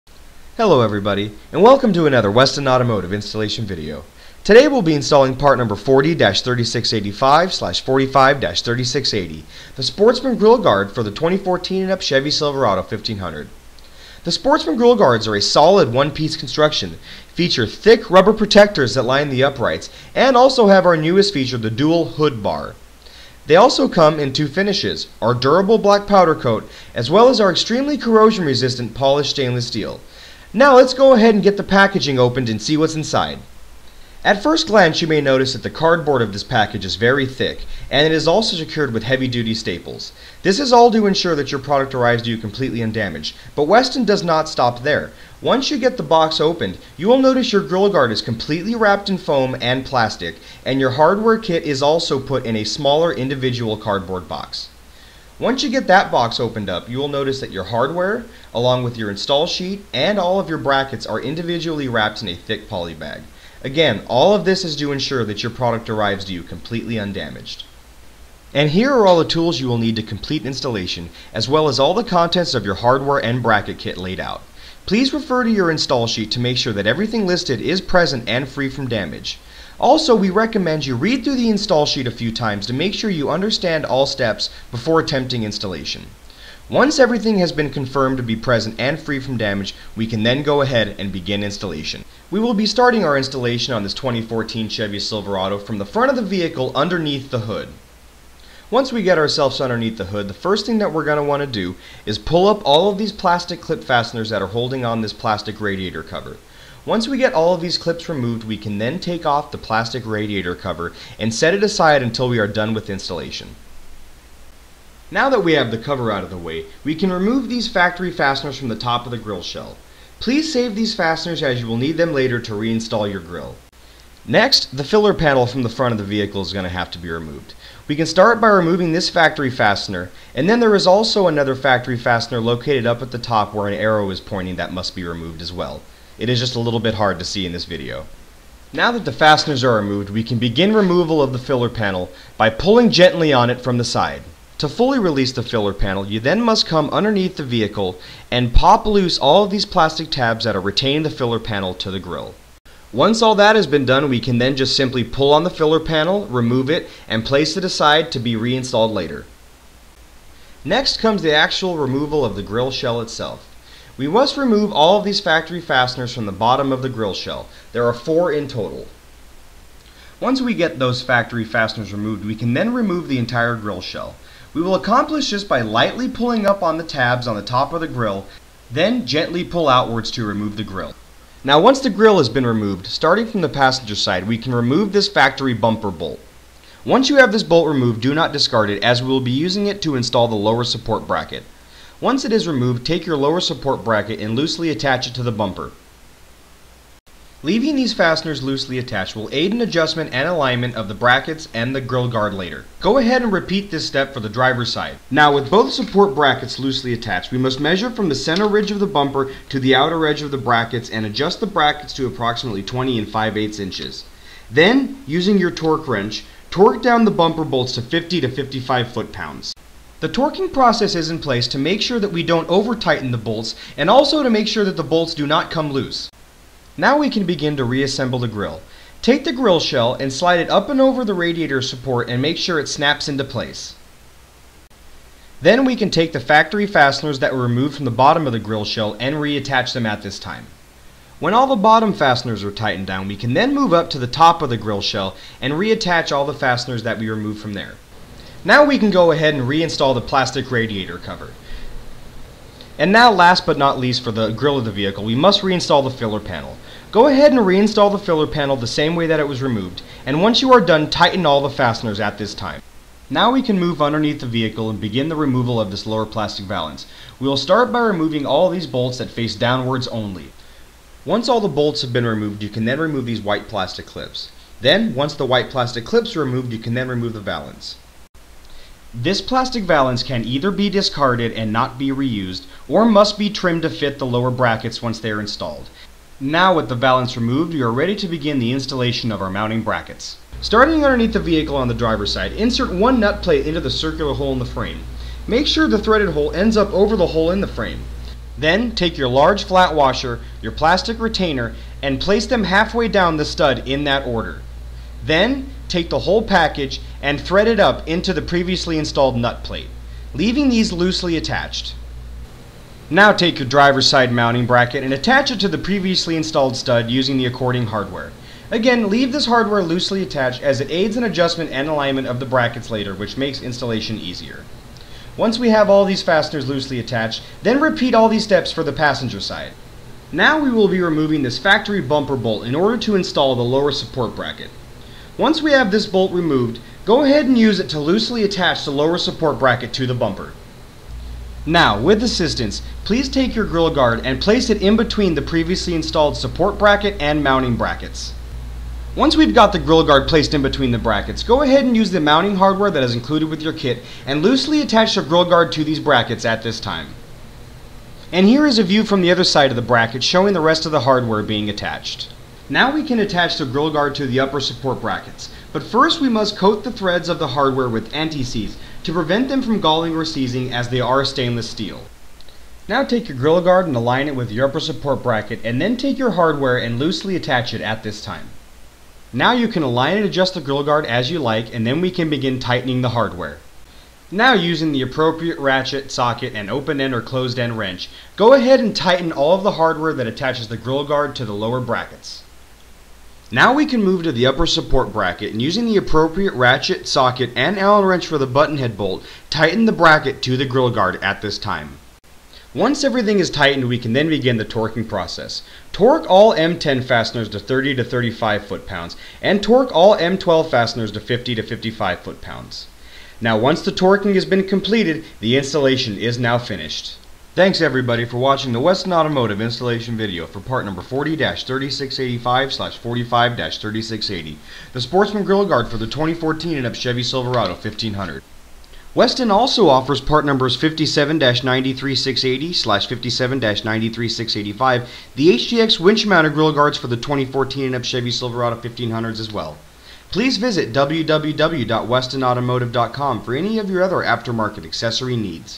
Westin 40-3685 Sportsman Grille Guard installation on 2014-2015 Chevy Silverado
Installation of Westin 40-3685 Sportsman Grille Guard with black powder coat finish for the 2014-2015 Chevy Silverado 1500. Also available in chrome plated stainless, part number 40-3680.